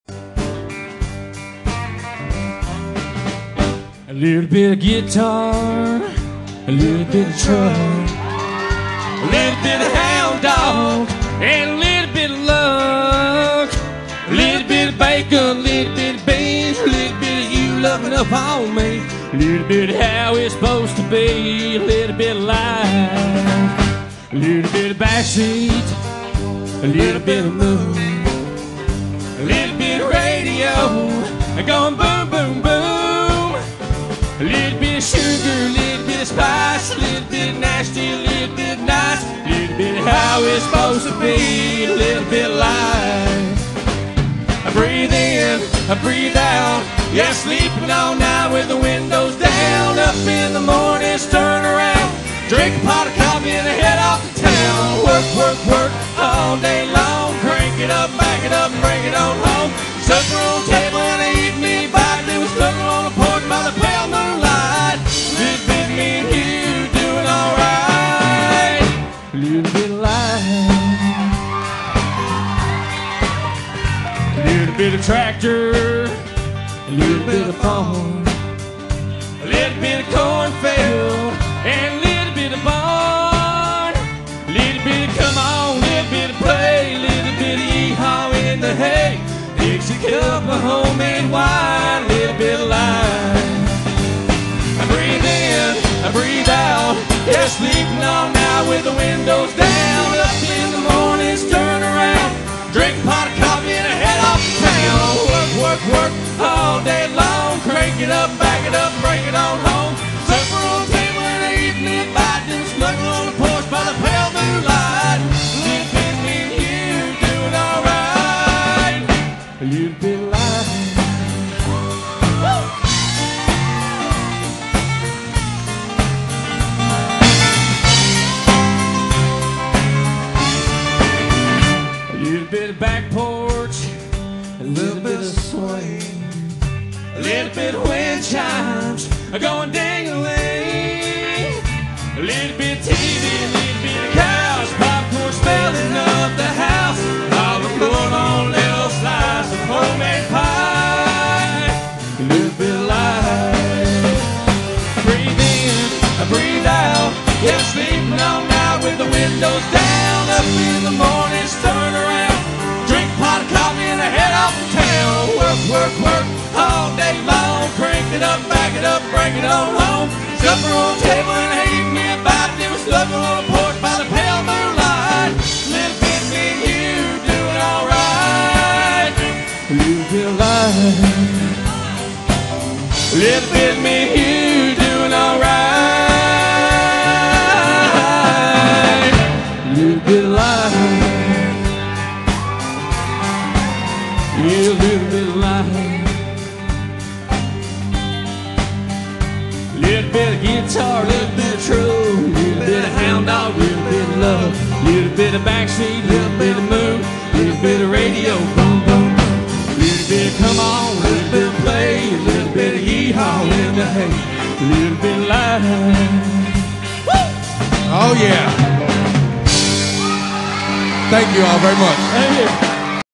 Josh & Jonathan Phelps in Top 4 Adult divison at Kentucky Opry
Josh & Jonathan Phelps made it into the Grand Finals on May 1st at the Kentucky Opry  Semi-finals on May 1st,2010.